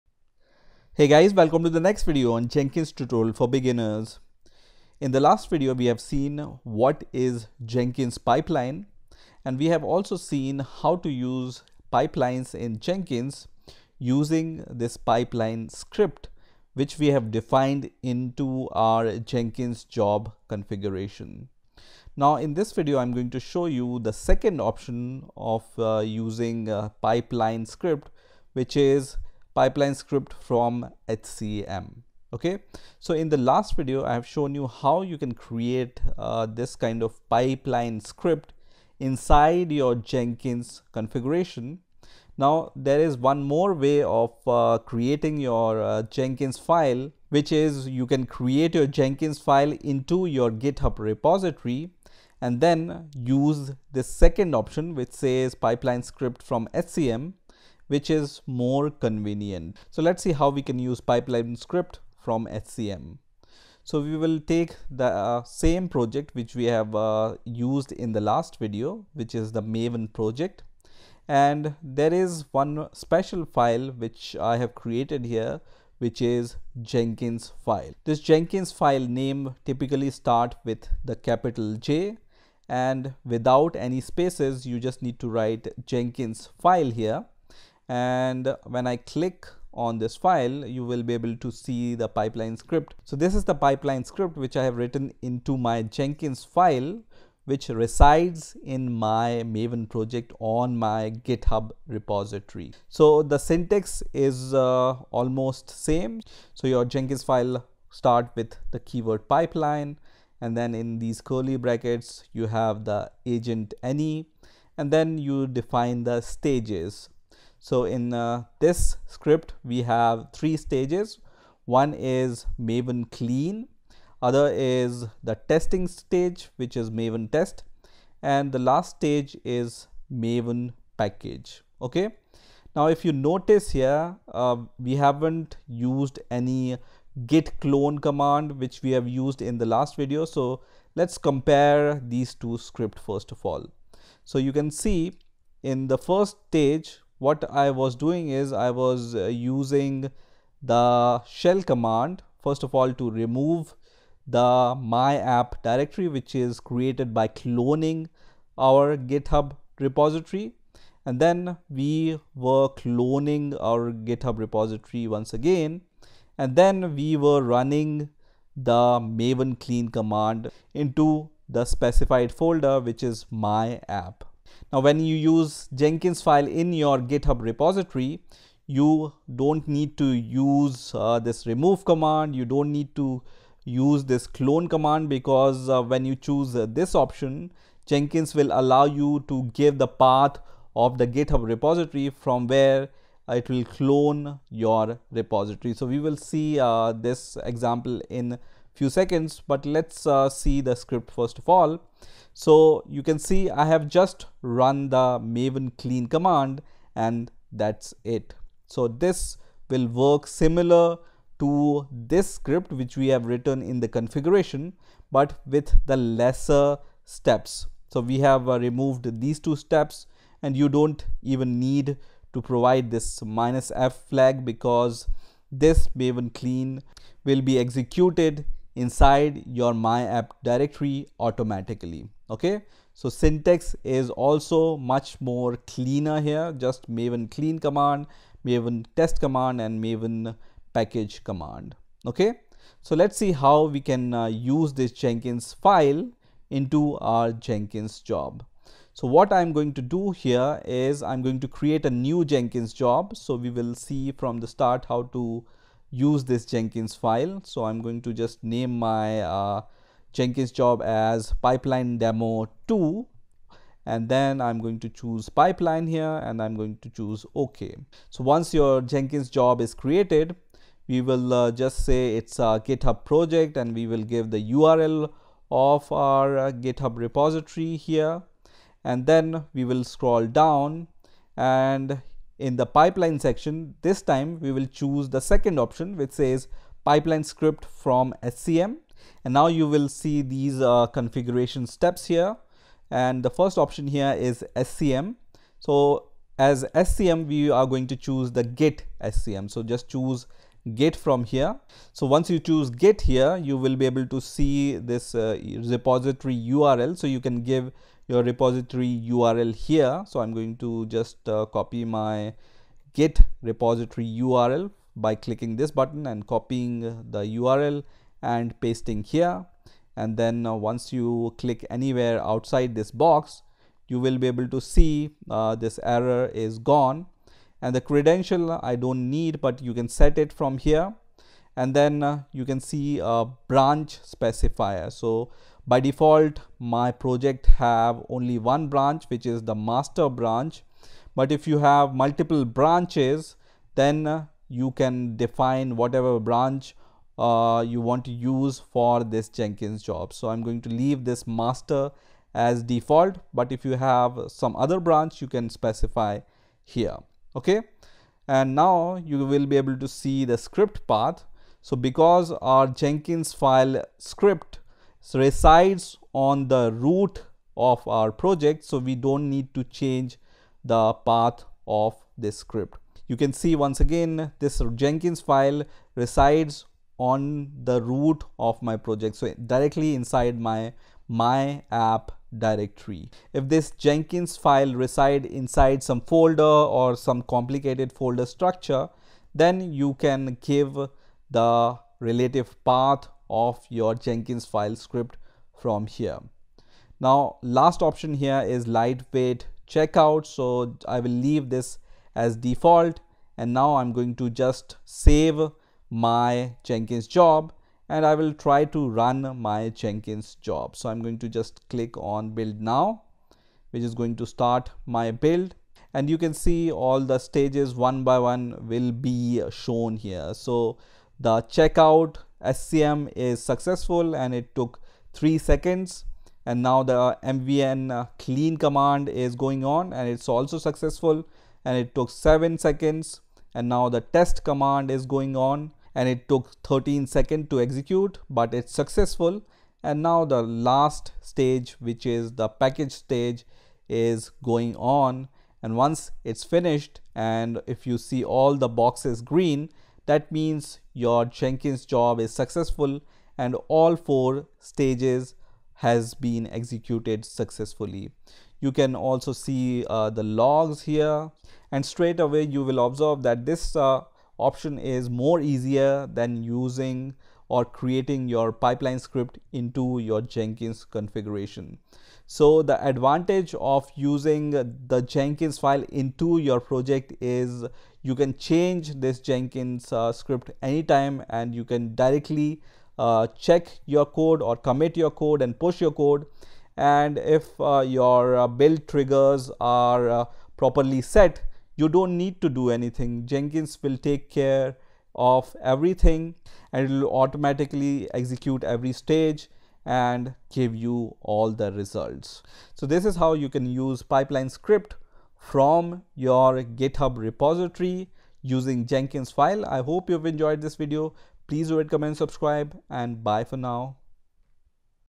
Jenkins Tutorial For Beginners 15 - Pipeline script from SCM + Using Jenkinsfile in Github Project
In this Video I am going to show How to use Pipeline script from SCM and How to use Jenkinsfile in Github Project.Jenkins Pipeline is a Whole suite of plugins (10+) which are open-sourced and Shipped with Jenkins 2.x.x.  These Pipeline plugins were Formerly commercially available by CloudBees, called Workflow. We use a file called Jekinsfile to Define pipeline as code (again Groovy DSL) stage("Hello") { echo "*Hello*" } stage("World") { echo "*World*" }

Jenkins sample pipeline script in Jenkinsfile
pipeline {
    agent any
    stages {
        stage('---clean---') {
            steps {
                sh "mvn clean"
        stage('--test--') {
            steps {
                sh "mvn test"
        stage('--package--') {
            steps {
                sh "mvn package"